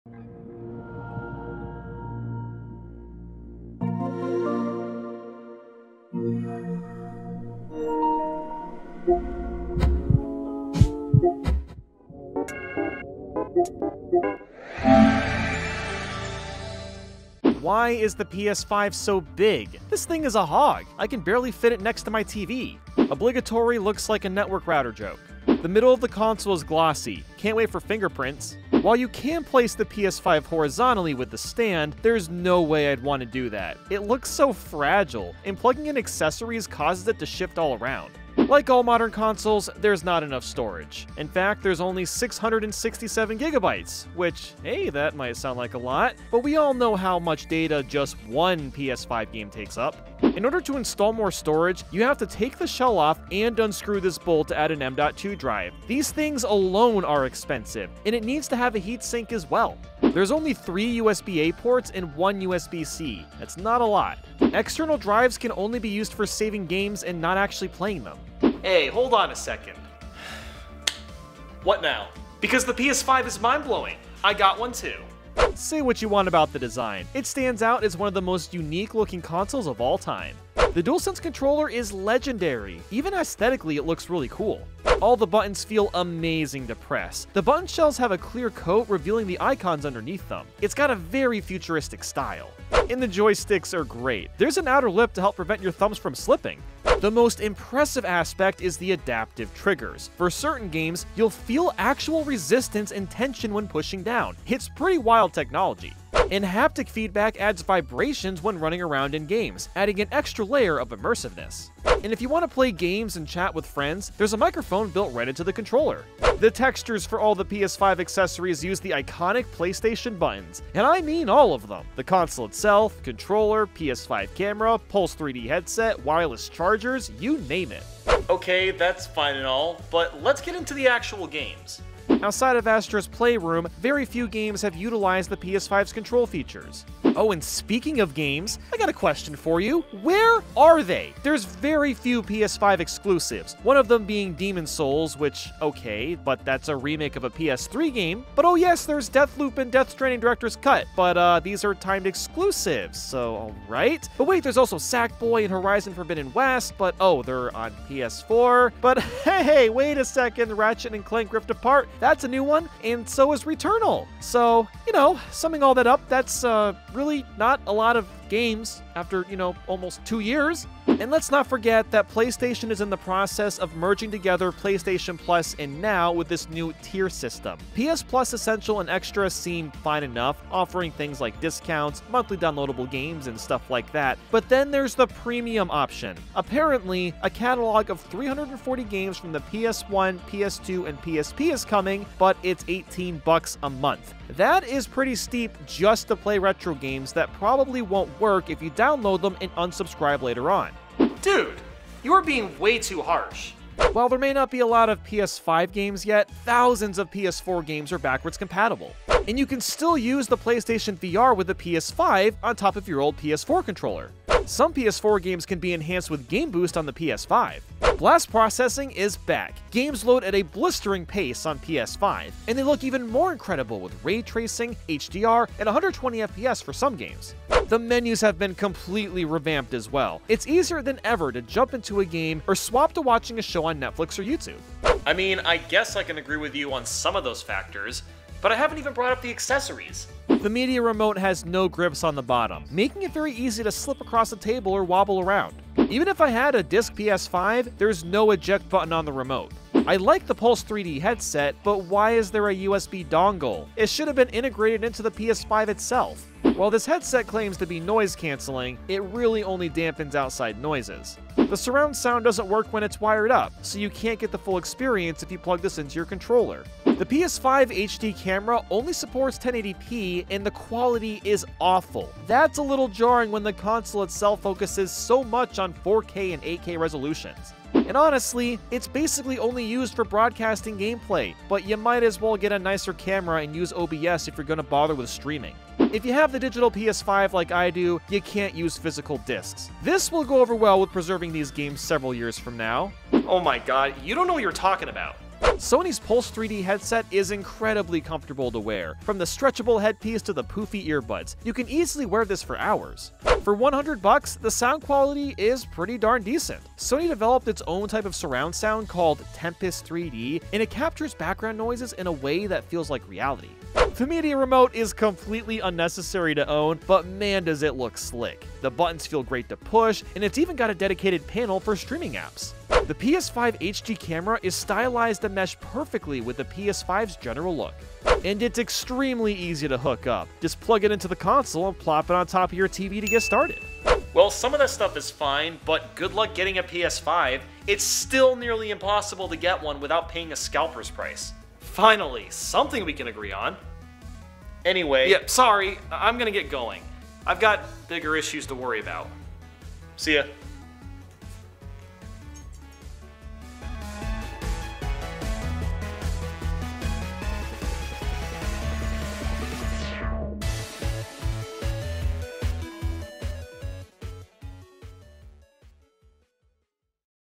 The Good and Bad of the Playstation 5
The Playstation 5 has been out for almost 2 years, and sometimes I still don't think it's real

Video Edited by: @TotallyNotJon_

Chapters:
Intro 0:00
Console 0:17
Games 2:12
Accessories 4:00
Outro 5:34